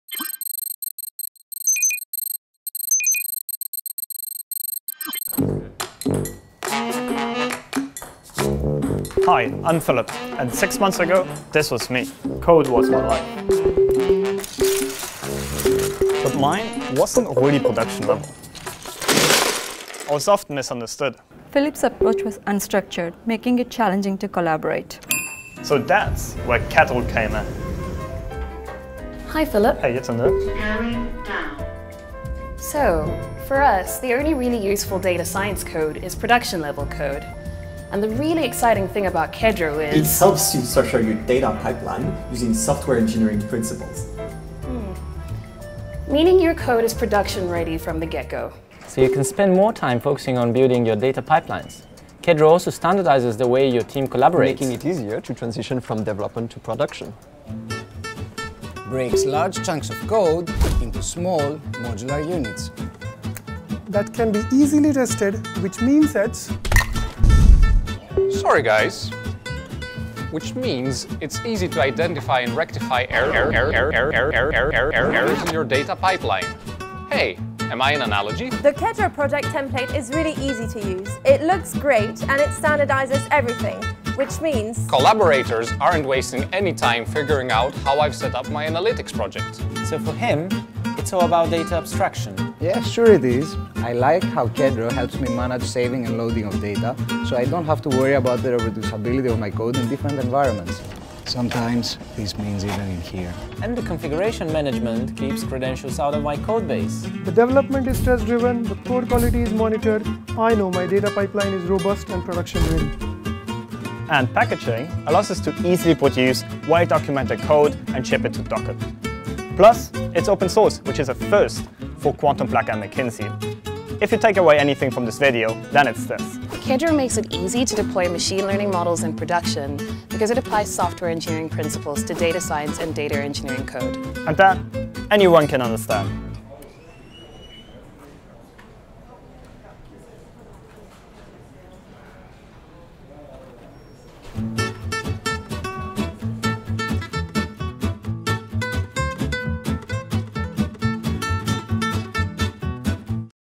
Introducing Kedro
Improved business performance is increasingly driven by Data Science and Machine Learning. For that reason, it is of crucial importance that the code powering key business decisions is deemed production quality. Machine learning models which can be deployed effortlessly and operate unattended are far more likely to achieve commercial objectives. At QuantumBlack, we've always asserted that the only useful data science code is production-level.

Learn more about Kedro, in this video created by Kedro users for Kedro users.